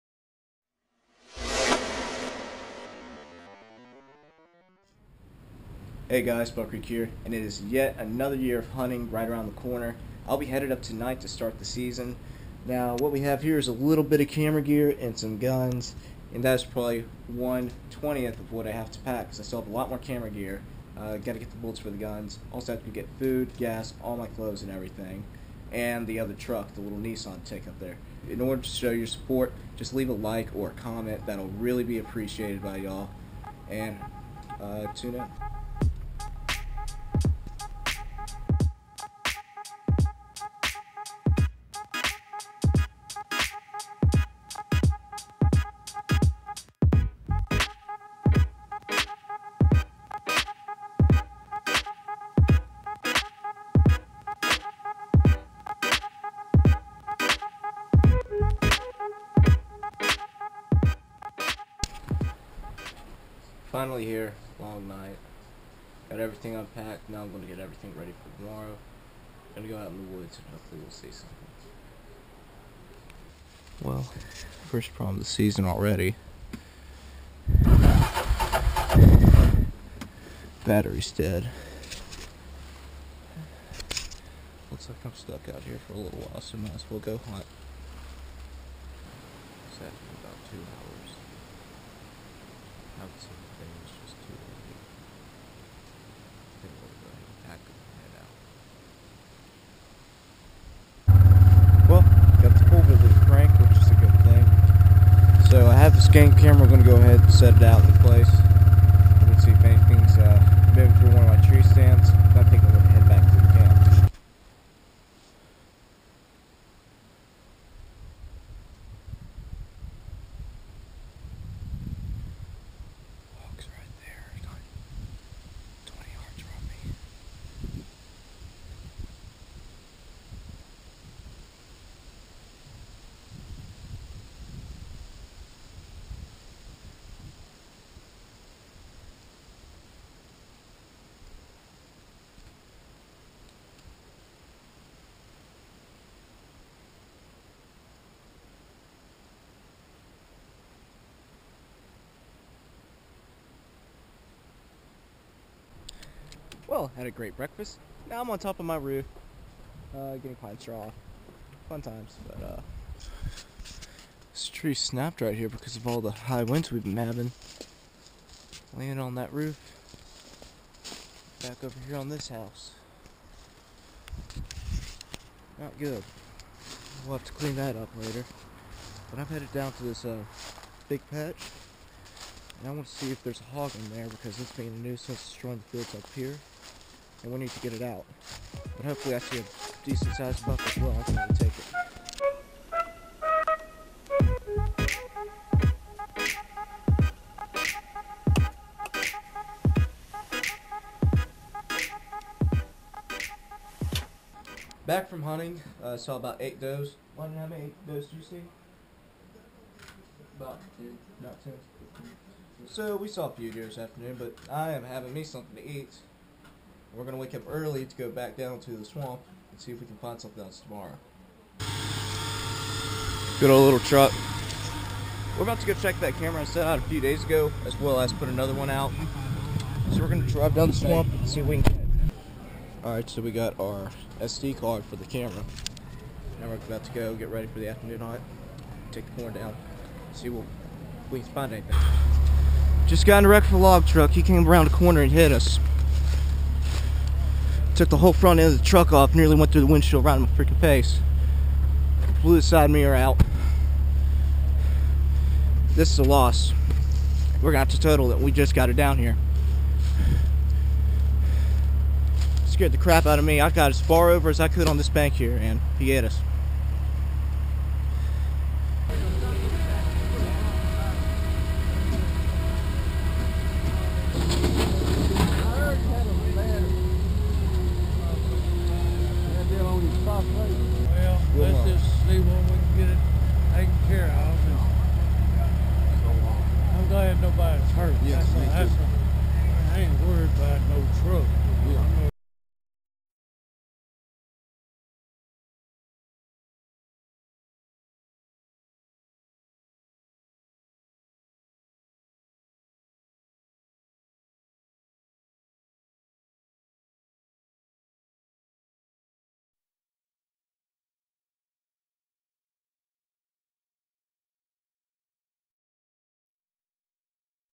Nissian Frontier in awful wreck
I went hunting the past few days and had an adventure from hogs and climbing on the roof to totaling the Nissan frontier.